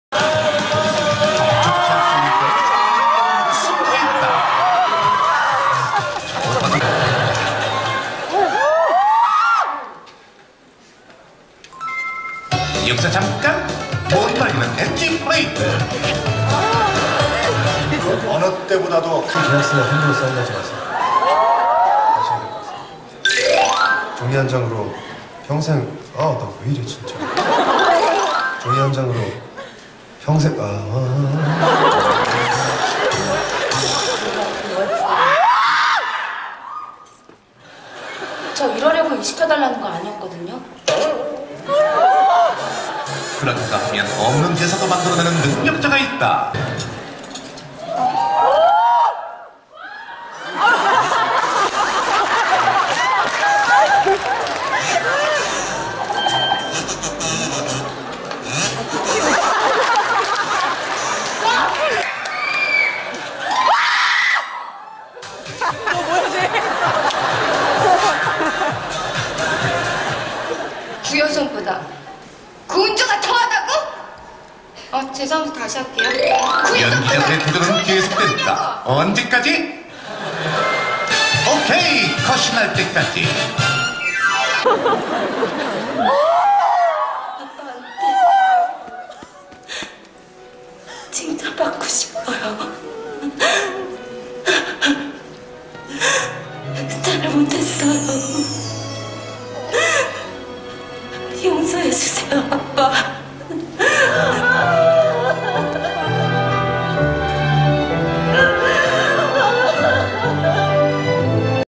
2010.12.31 화면 감상중 (연기대상 문근영 직캠 )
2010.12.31 KBS연기대상 문근영 직캠
연기대상 화면보며 리액션中 Cam by 죠리퐁짱팬